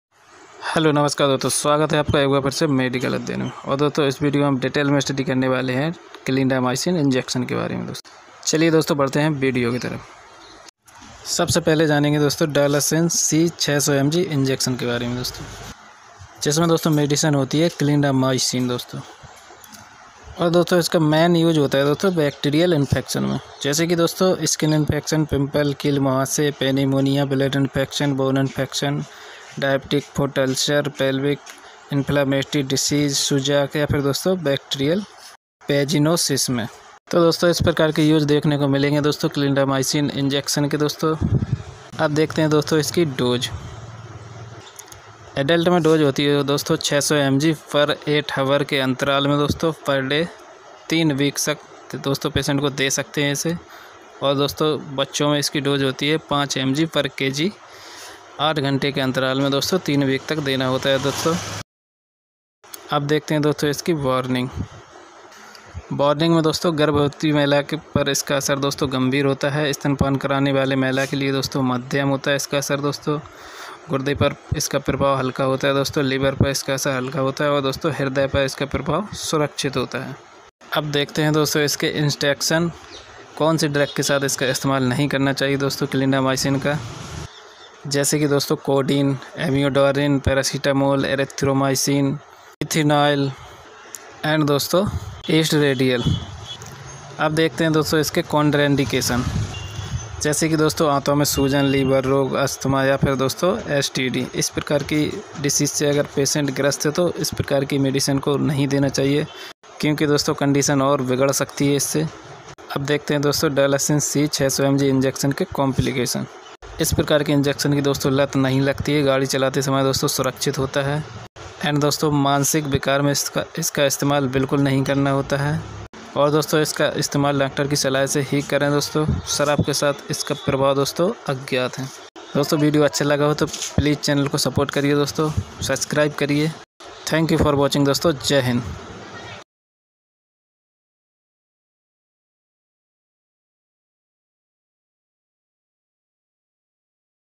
Clindamycin injection/use/ कब और कहां करें पूरी जानकारी/Dalacin c 600mg injection/use/Dose/warning".
Hello friends 🙏🙏🙏👍👍🌹

This video make only for Dalacin c 600 mg
injection full knowledge..
Like use Dose warning side effects..

Medical Adhyayan 👍👍👍🙏🙏🙏

This video is purpose only for Medical knowledge
Please consult a Doctor before using any medications